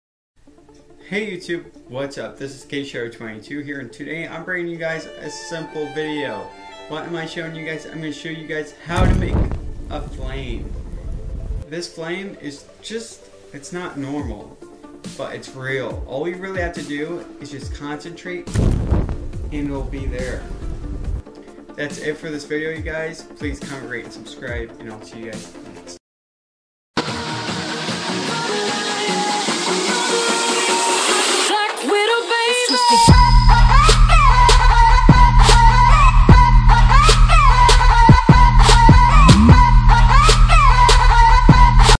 How To Make Fire
This was produced with Adobe After Effects, This is also a animation.